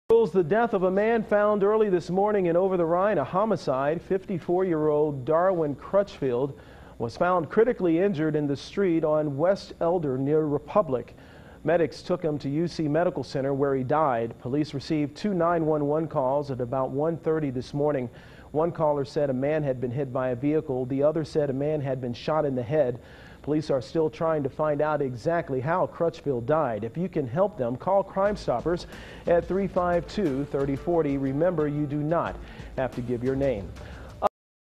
Homicide Investigation Continues
CINCINNATI (WKRC) -- Cincinnati Police are continuing to investigate the death of a male early Friday morning in Over the Rhine.Darwin Crutchfield was found in the roadway on 16th and West Elder and transported the UC Hospital where he later died.  The Hamilton County Coroner has ruled the death a homicide.He was 54 years old.Anyone with information about this homicide is asked to call Crime Stoppers at 352-3040.